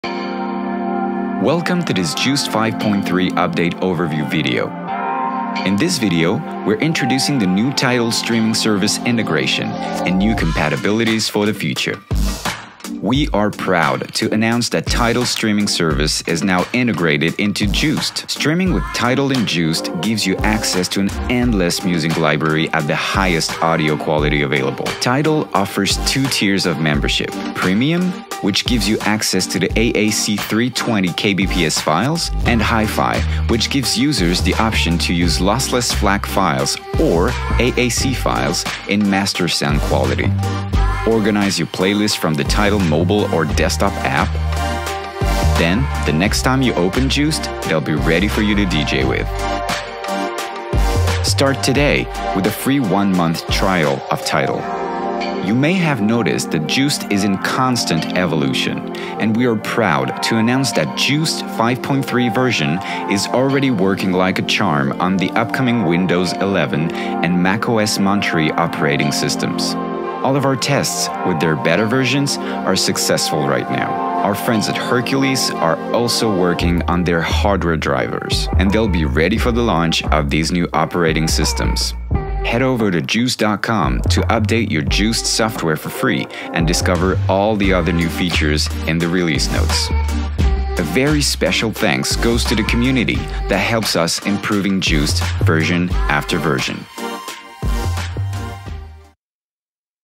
DJUCED | Autumn 2021 Update (version 5.3)
Welcome to this DJUCED Update overview video.

In this video, you will discover the new DJUCED 5.3 features such as:
00:00 Intro
00:13 Tidal integration
00:55 Windows 11 & macOS Monterey compatibility
01:22 Outro - Thank you!